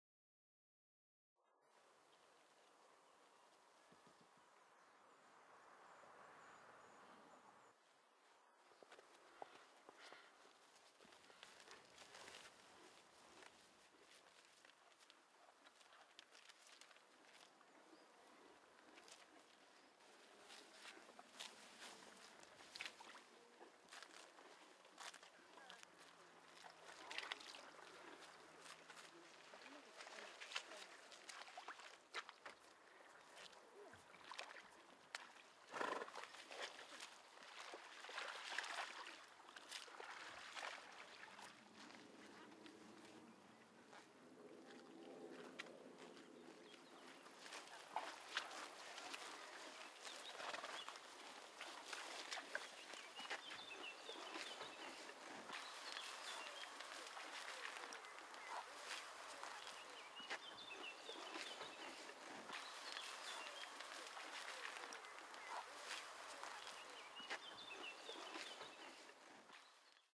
Large herbivores: young Konik stallions follow the herd
Looking for footage? Don't shoot! Lot of shots are at your disposal.

The younger Konik stallions follow the herd  in Blauwe Kamer nature reserve. In herds, there is usually a single lead stallion, though occasionally a few less-dominant males may remain on the fringes of the group.
Every herd has leaders, followers, and a well defined pecking order known as a dominance hierarchy.